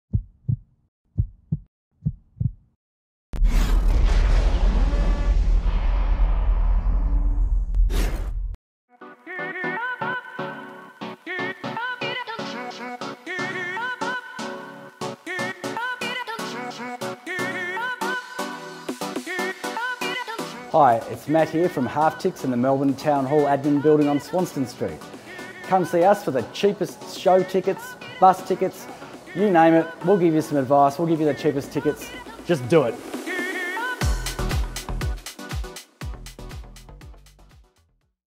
Ticket Service | Half Tix | Ticket Sales | Melbourne | VIC | Review | Content
Come and visit Australia's longest running discount ticket service! Halftix Melbourne specializing in live shows and tours, they have the widest range of events at the best prices in town.

Location: Admin Building, Melbourne Town Hall, 90-120 Swanston Street Melbourne VIC 3000